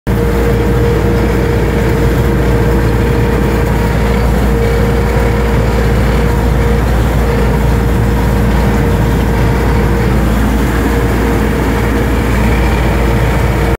Rzepak 4.4 tony claas consul
A ja dalej walcze 49 letnim clasikiem consulem bez stolu 0.8  hektar na godzine